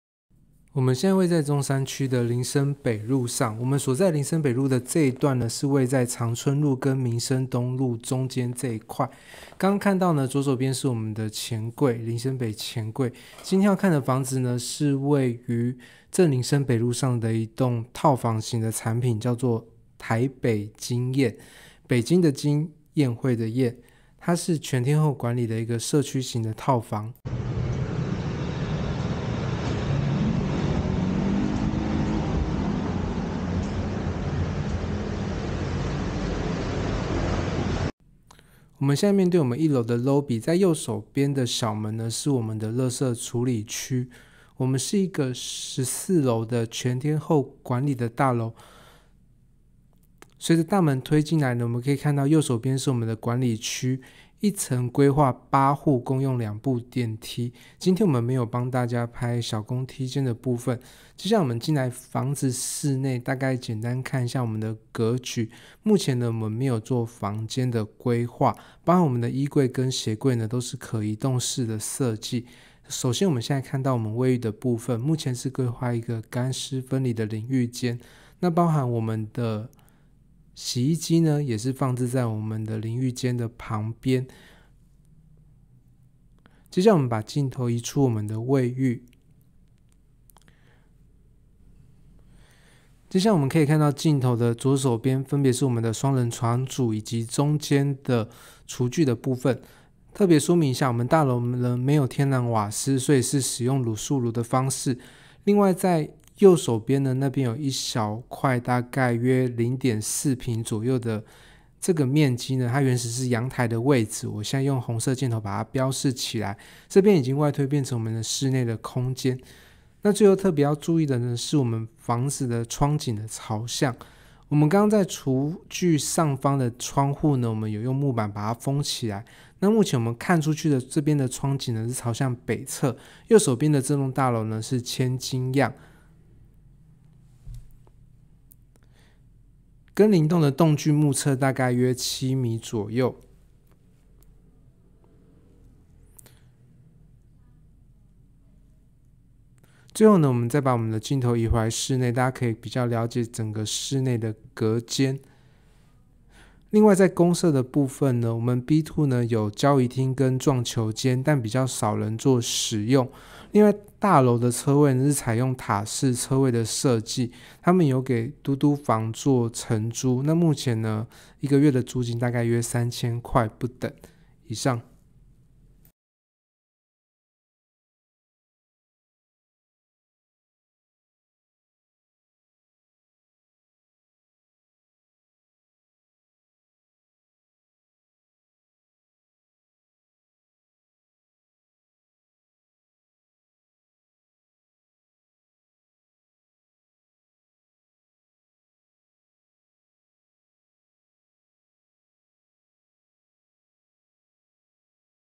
中山區 林森北路305/13-6
00:00新生北路二段62巷口
00:38社區一樓
00:57進入房屋室內
02:05陽台窗景(朝北)

社區名稱 :台北京宴
房屋型態:電梯大樓
所在樓層:14樓/14樓蓋

建物權狀:13.52坪
主建物:8.65坪
附屬建物:0.44坪
公設:4.43坪
車位類型/坪數:；坪

土地分區/持分/基地:商四特(原商三)；1.49坪；225.67坪
格局(房/廳/衛/陽台):0/1/1/0
學區:吉林國小；新興國中
使照用途/高度:集合住宅(92使字63號)；3.25米
管理時間:全日
一層戶數:8戶
垃圾處理:一樓側邊空地
捷運google步行時間:
補充:
公設B2有交誼廳撞球間 但因疫情關係 已很少人使用
車位部分已滿 大樓有跟嘟嘟房合作 租金3000元/月

喬信不動產仲介經紀有限公司
經紀人:唐鉅棟(103)北市經證字第02130號